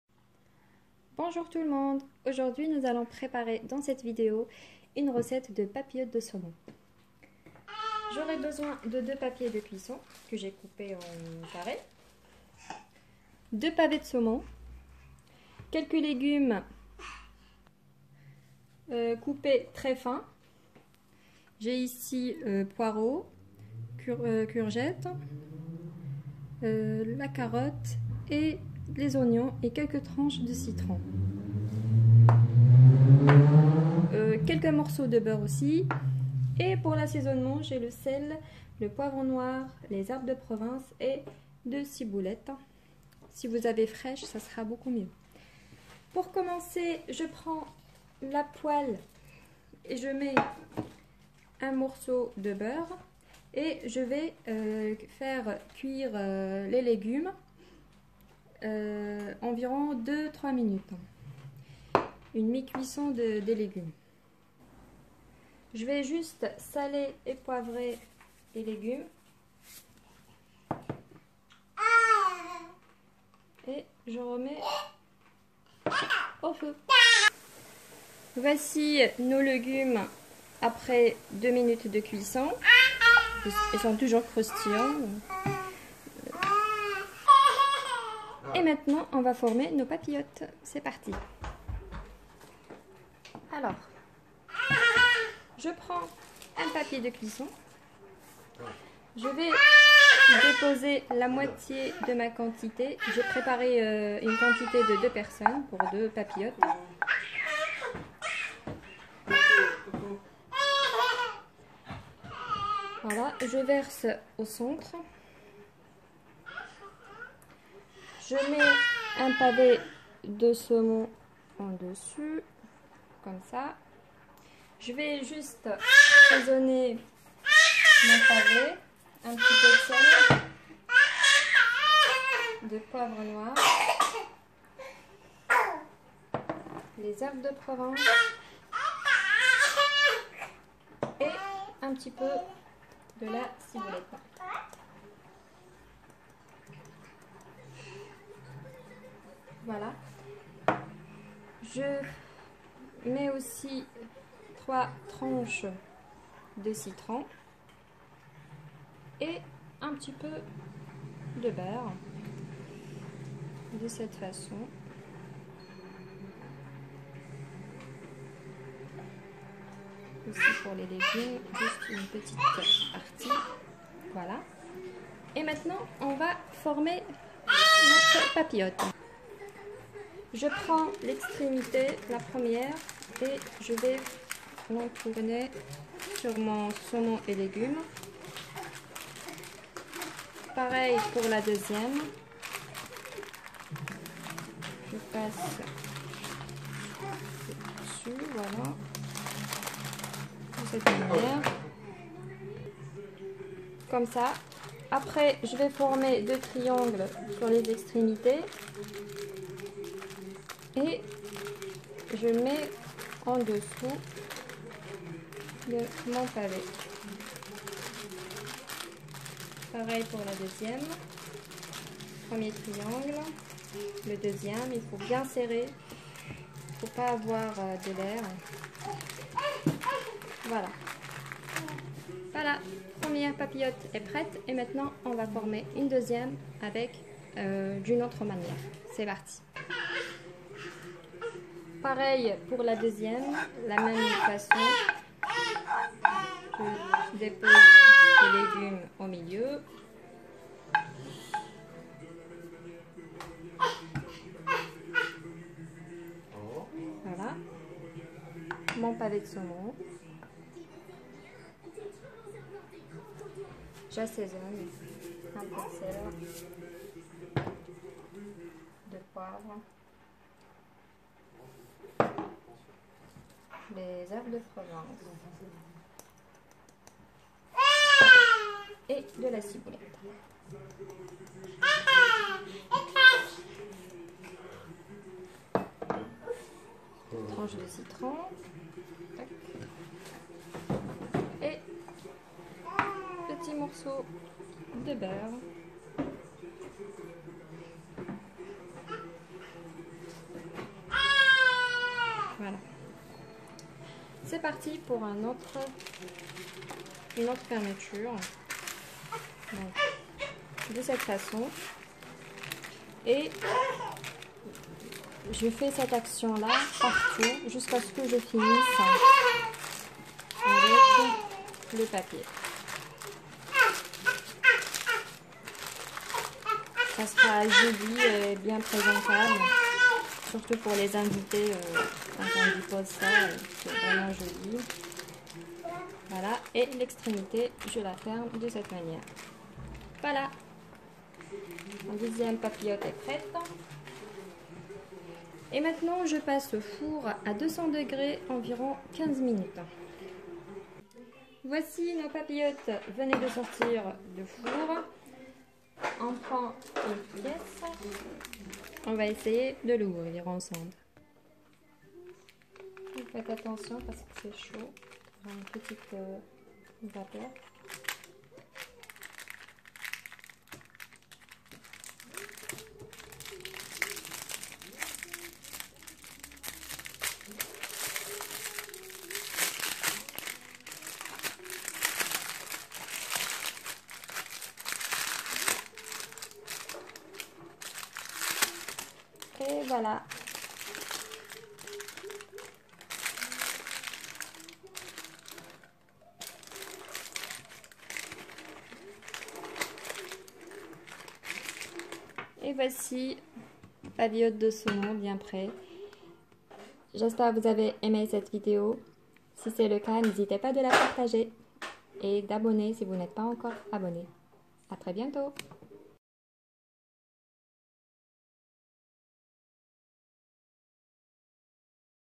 Papillote de saumon. Facile et rapide
pavée de saumon en papillote très simple à réaliser et excellente pour votre santé puisque la cuisson en papillote conserve le goût et les vitamines. Je vous la conseil.

les ingrédients :
Pavée de saumon
Carotte coupée très fine
Courgette coupée très fine
Poireaux coupé très fin
Oignon  coupé très fin
Quelques tranches de citron
Sel, poivre, herbes de Provence, ciboulette
Quelques morceaux de beurre
Papier de cuisson